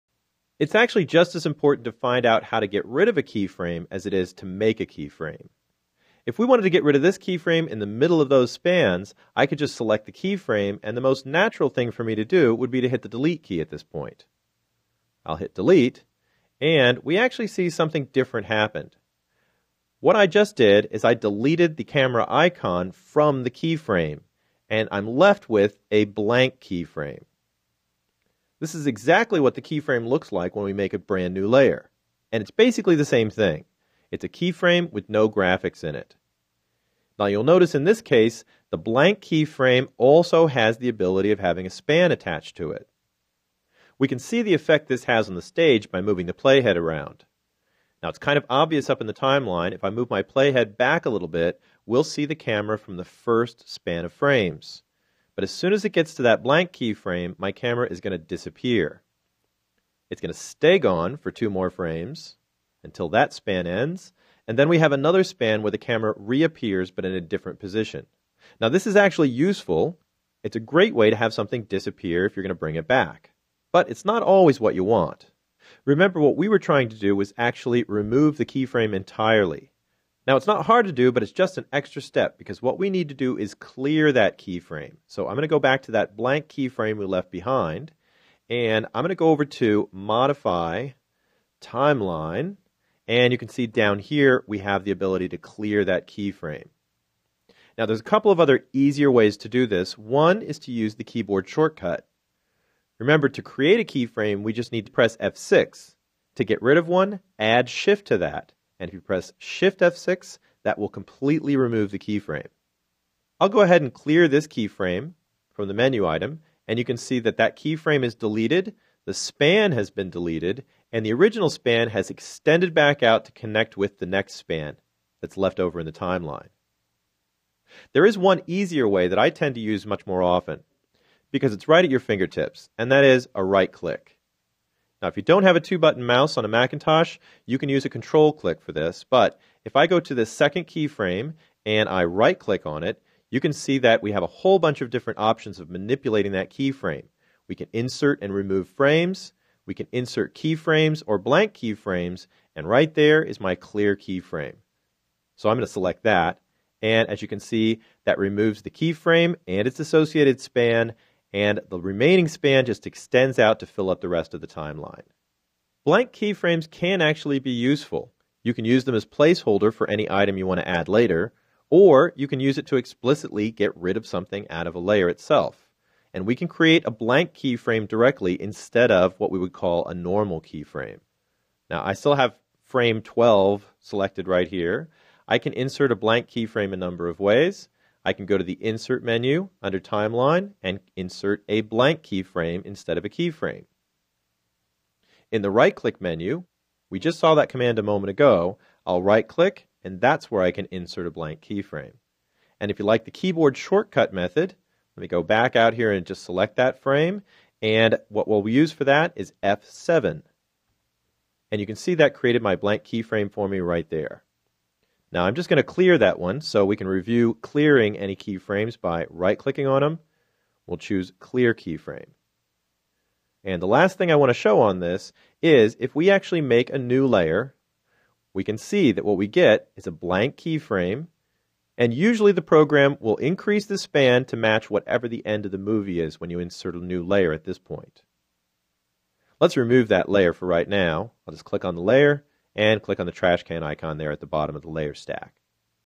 How to Insert Blank Keyframes & Clear Keyframes In Adobe Flash
Chapter 3
-THE TIMELINE - Lesson 4
-Insert Blank Keyframes & Clear Keyframes Tutorial